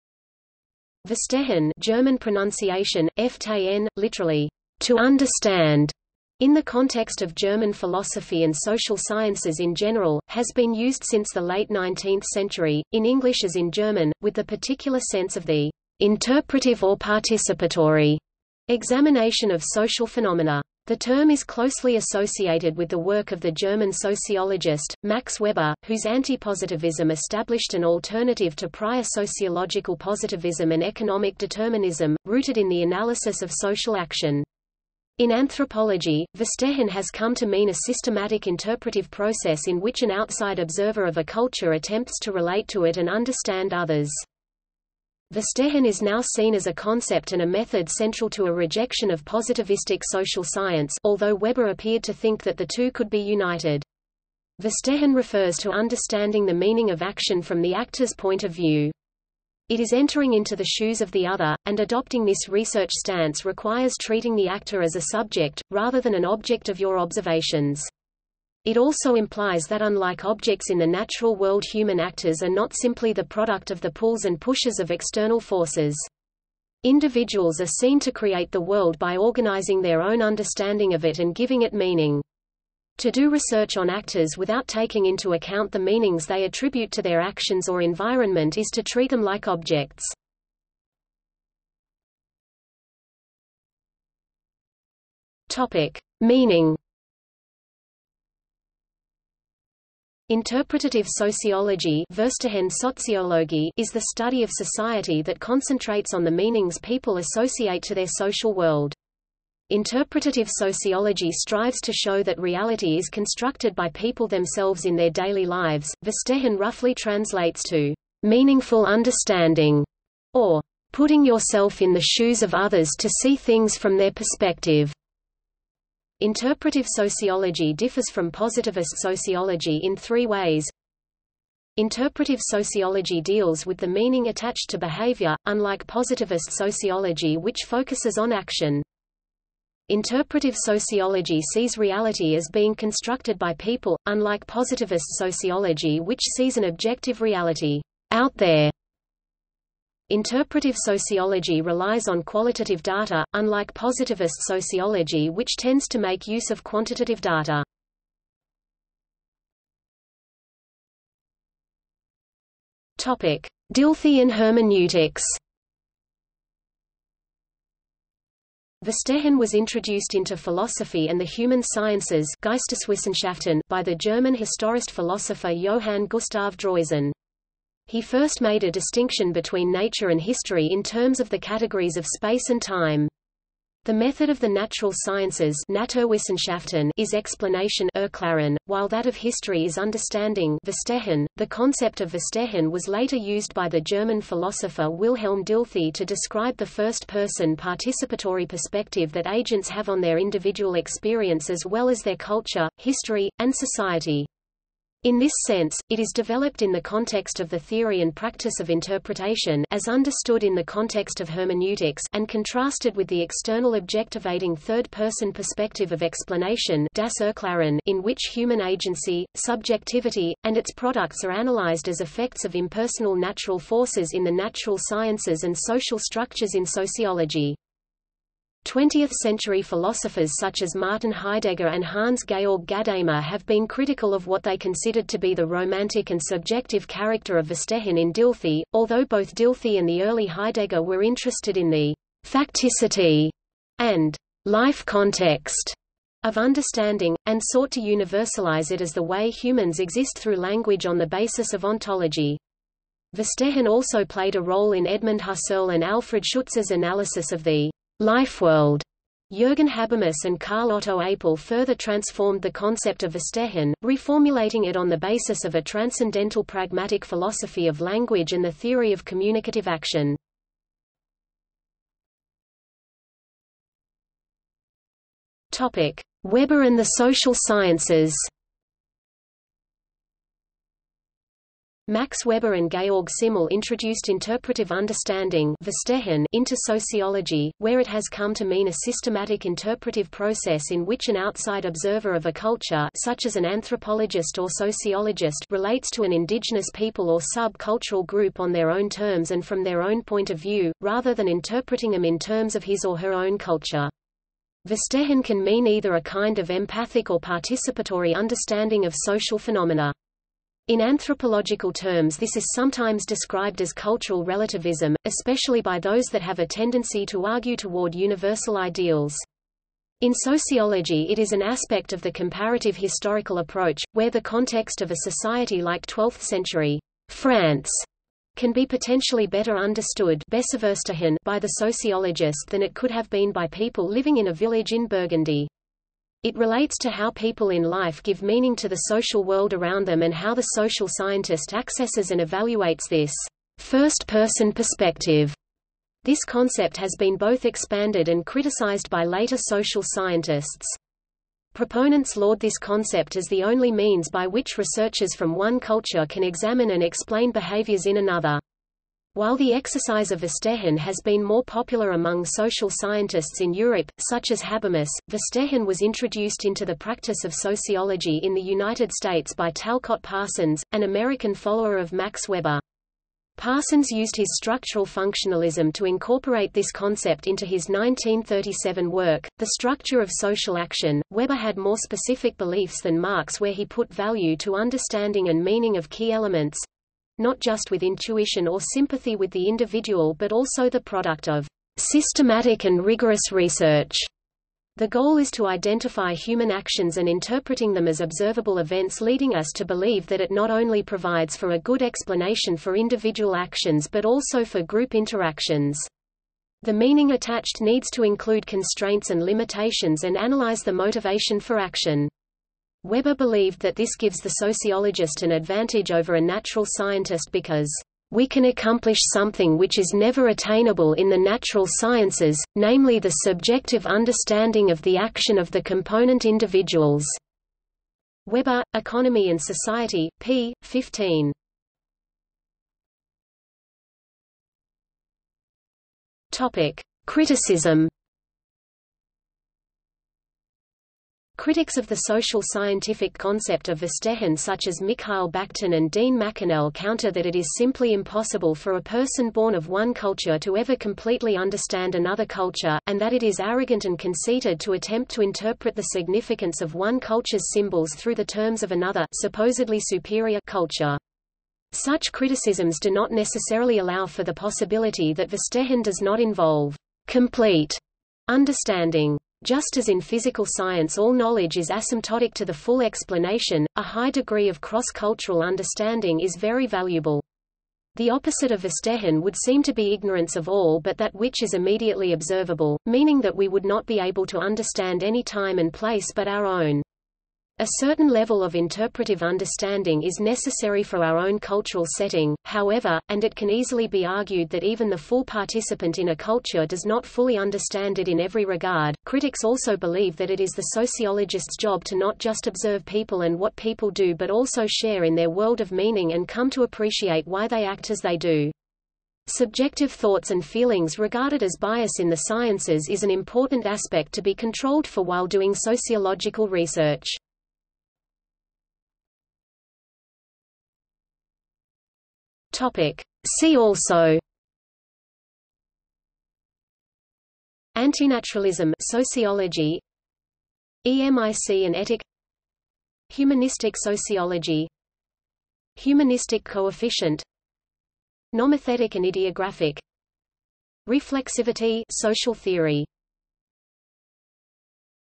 Verstehen | Wikipedia audio article
00:01:32 1 Meaning
00:02:39 2 Dilthey and hermeneutics
00:04:32 3 Weber and the social sciences
00:07:12 4 Criticism
00:08:39 5 See also

- Socrates

SUMMARY
Verstehen (German pronunciation: [fɛɐˈʃteːən], literally: "to understand"), in the context of German philosophy and social sciences in general, has been used since the late 19th century – in English as in German – with the particular sense of the "interpretive or participatory" examination of social phenomena. The term is closely associated with the work of the German sociologist, Max Weber, whose antipositivism established an alternative to prior sociological positivism and economic determinism, rooted in the analysis of social action. In anthropology, Verstehen has come to mean a systematic interpretive process in which an outside observer of a culture attempts to relate to it and understand others.
Verstehen is now seen as a concept and a method central to a rejection of positivistic social science (although Weber appeared to think that the two could be united). Verstehen refers to understanding the meaning of action from the actor's point of view. It is entering into the shoes of the other, and adopting this research stance requires treating the actor as a subject, rather than an object of your observations. It also implies that unlike objects in the natural world human actors are not simply the product of the pulls and pushes of external forces. Individuals are seen to create the world by organizing their own understanding of it and giving it meaning. To do research on actors without taking into account the meanings they attribute to their actions or environment is to treat them like objects.